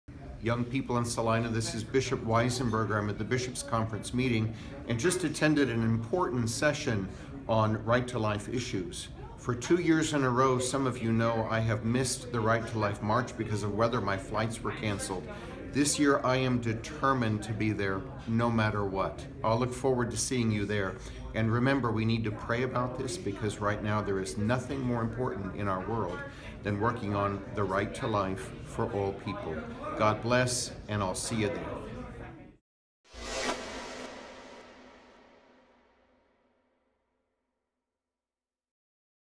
US Bishops For Life: Weisenburger
Join Bishop Weisenburger today to support life and liberty of conscience. Be a voice for the voiceless. Together, we will be heard!